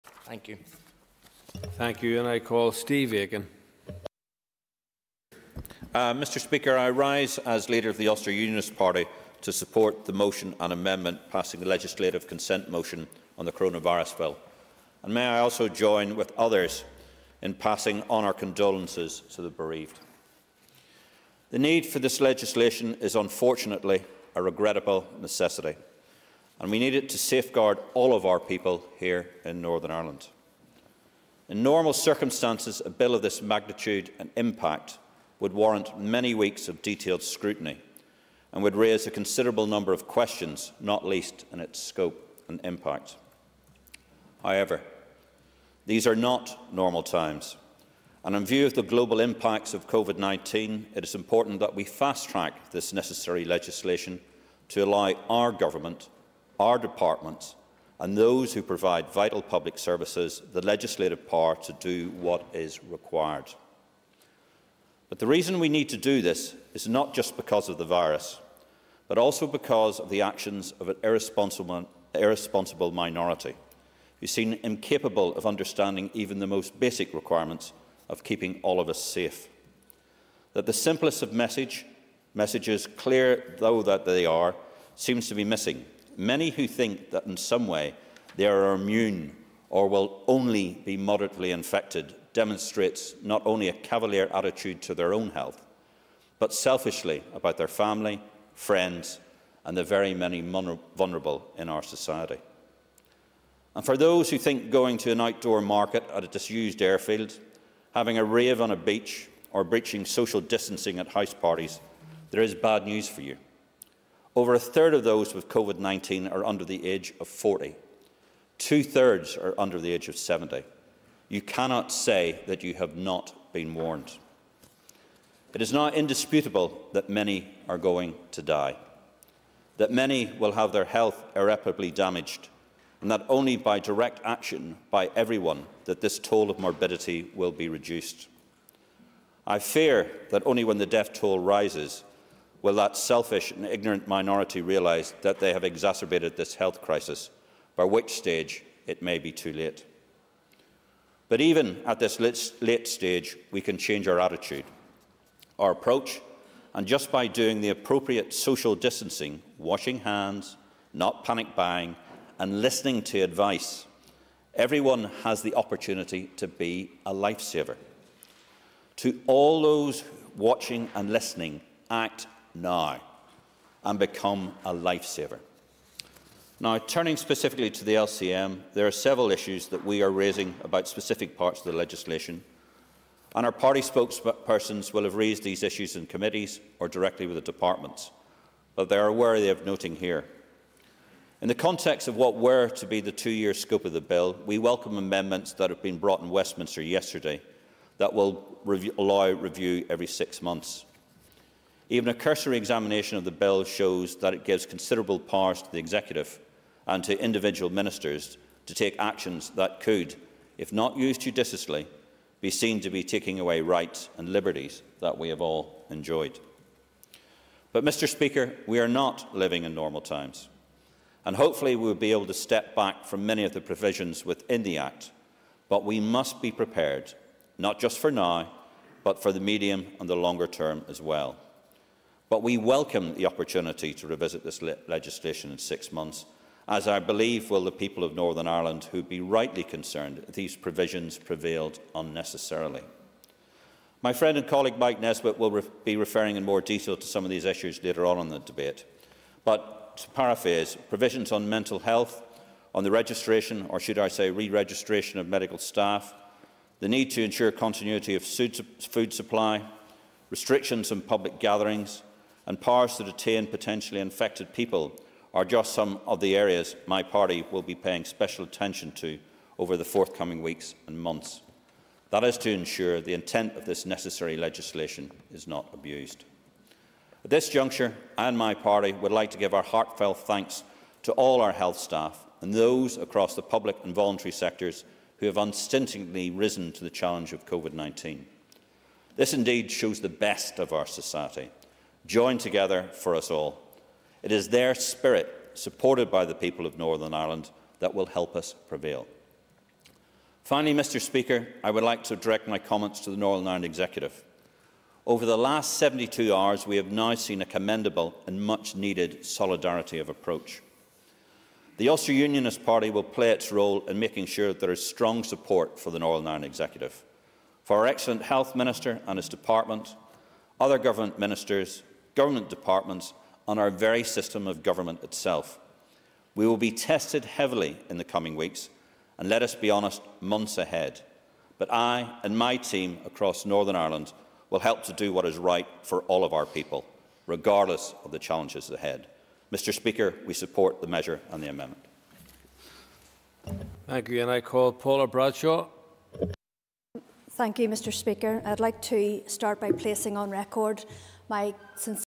Legislative Consent Motion on the Coronavirus Bill: Steve Aiken
Steve Aiken, leader of the Ulster Unionist Party, speaks on the Legislative Consent Motion on the Coronavirus Bill.